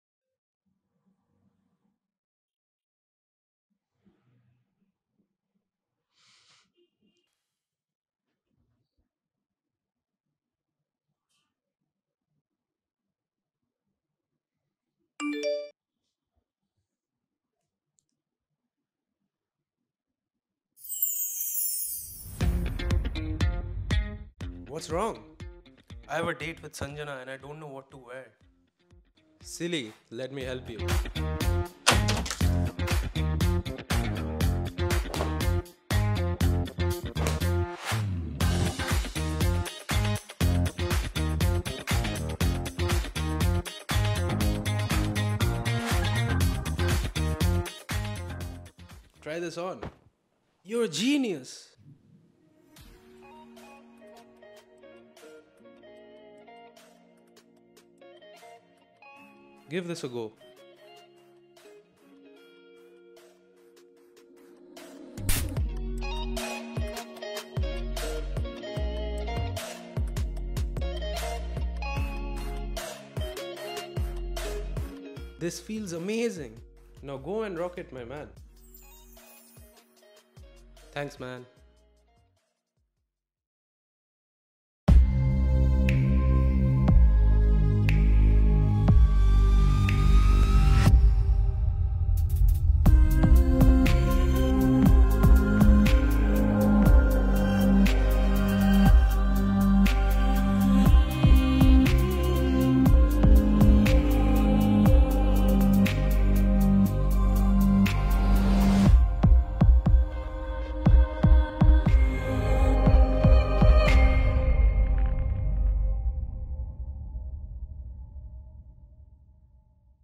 ELEVATE | A PRODUCT COMMERCIAL | Try the Titan Skinn Product Now!
Do you want to look your best? Elevate is here to help! In this product commercial, we see the amazing results that can be achieved when you use the Titan Skinn product. Watch this commercial today and see how to transform yourself for a date.

Elevate is a product commercial which I did for a college assignment. Had an awesome time filming this with my brother. It was a great learning experience too. I tried out different effects to incorporate in the ad.  Do let me know your thought in the comments!

Shot on Sony Alpha III and edited on Premiere Pro with my customized LUTs.